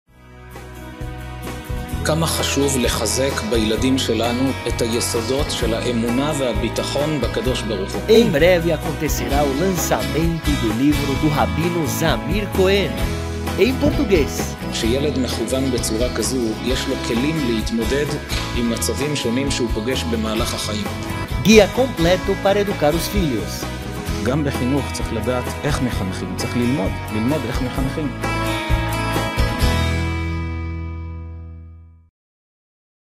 Lançamento! O livro do Rabino Zamir Cohen: Guia para educar filhos - em português! Hidabroot Brasil
*Lançamento!*
*Editora Hidabroot Brasil apresenta*
O livro do Rabino Zamir Cohen:
*Guia para educar filhos*
- em português!